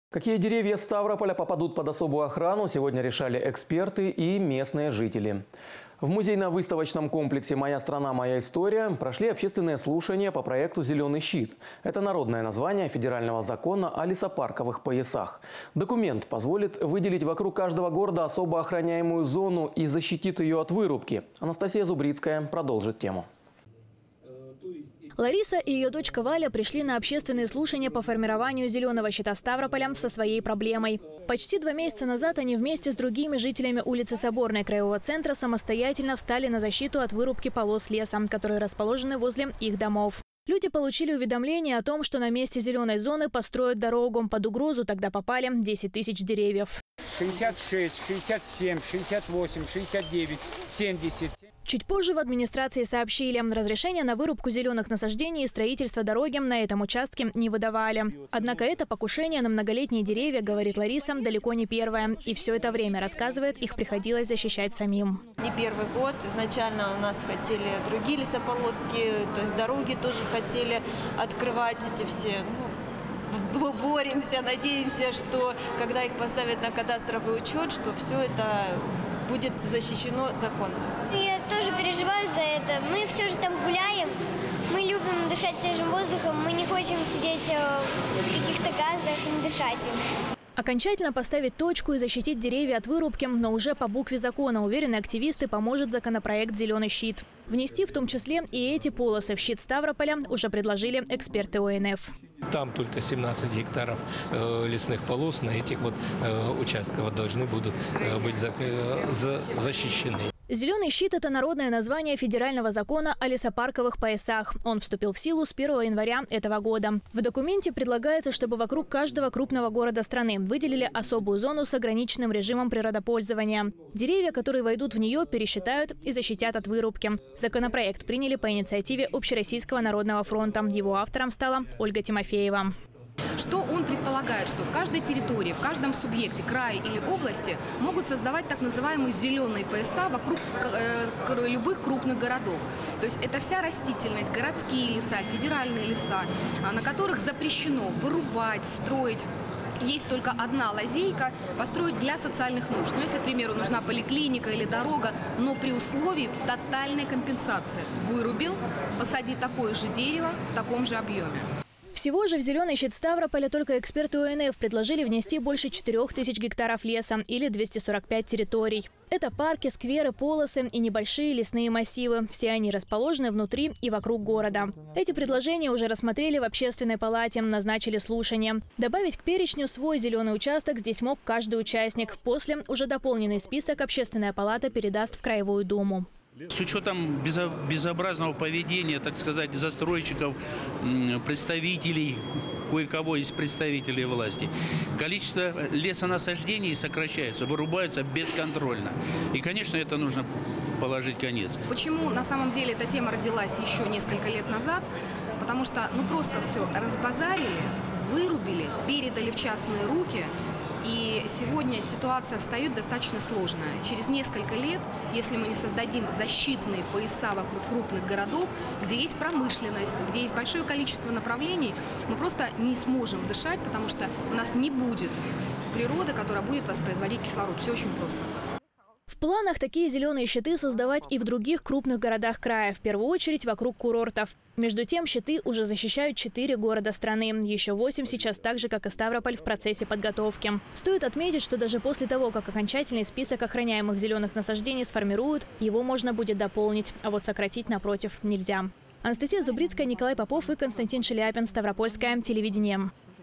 В Ставрополе выбирали какие деревья станут "Зелёным щитом" города
Какие деревья Ставрополя попадут под особую охрану сегодня решали эксперты и местные жители. В музейно -выставочном комплексе "Моя страна. Моя история" прошли общественные слушанья по проекту "Зелёный щит". Это народное название федерального закона о лесопарковых поясах. Документ позволит выделить вокруг каждого города особо охраняемую зону и защитит её от вырубки. Анастасия Зубрицкая продолжит тему.